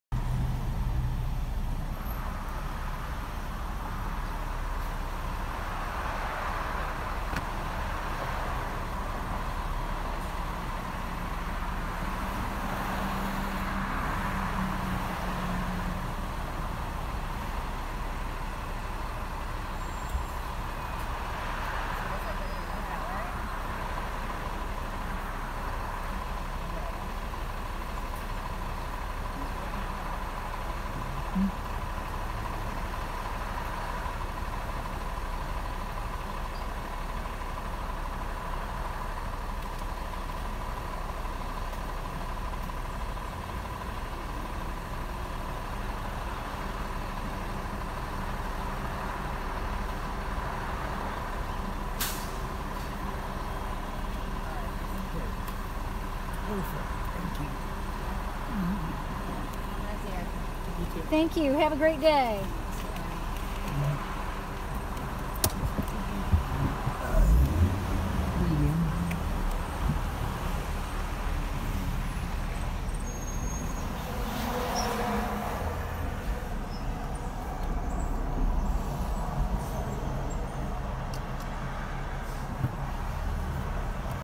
Kathy Jean
My production assistant, still photographer, and I were in the Dunkin' Donuts drive-thru. "Cars and Coffee Movie"